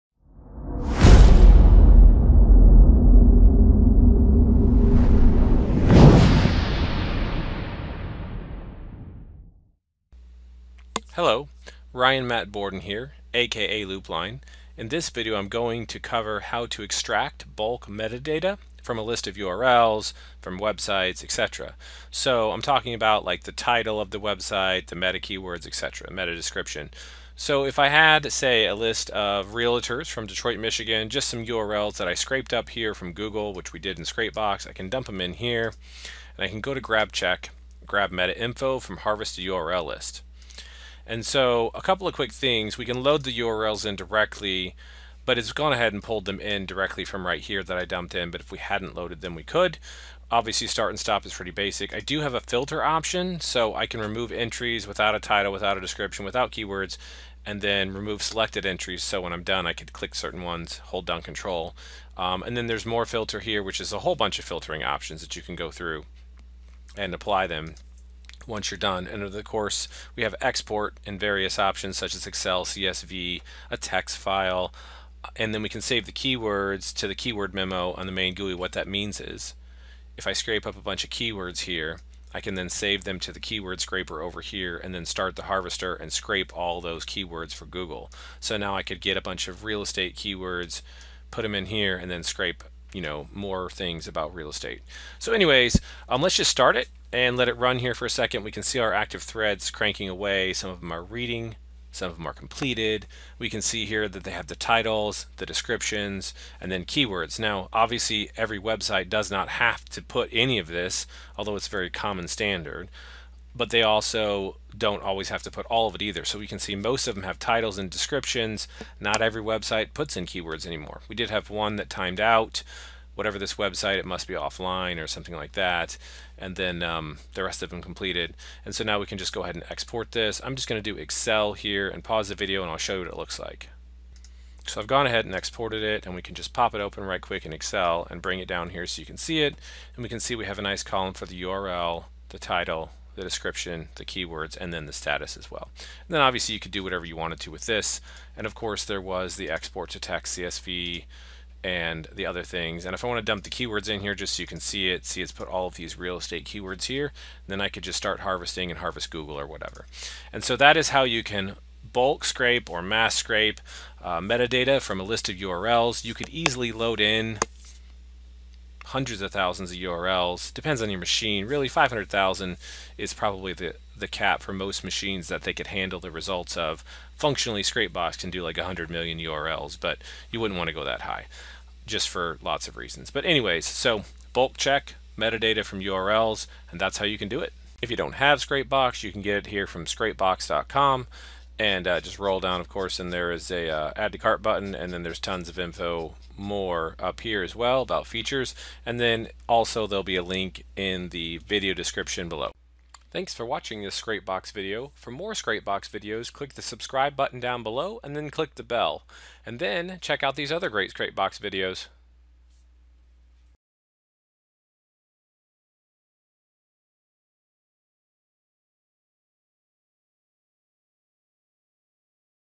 Extract bulk metadata from website url with Scrapebox
Extracting meta data from large lists of websites can be easily done with scrapebox usign the scrapebox meta data grabber.  You can easily work with lists of hundreds of thousands of urls at a time.  Proxies are supported, but not necessarily required.